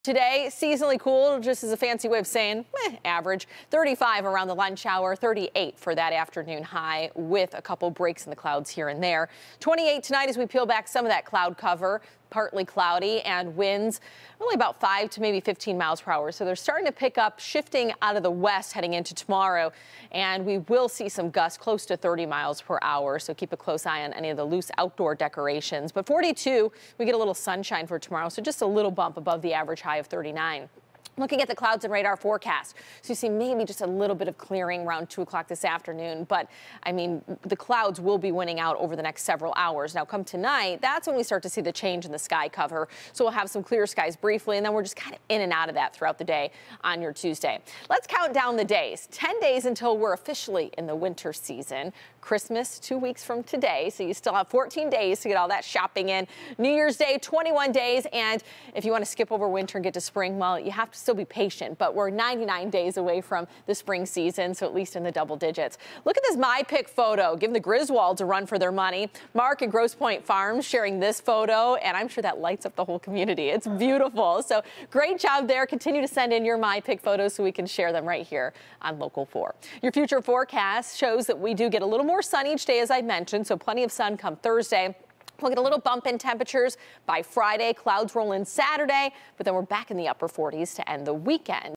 When to expect sunshine this week in Metro Detroit
We've got a mild week of weather ahead. We'll have clouds most days, but some sunshine is expected.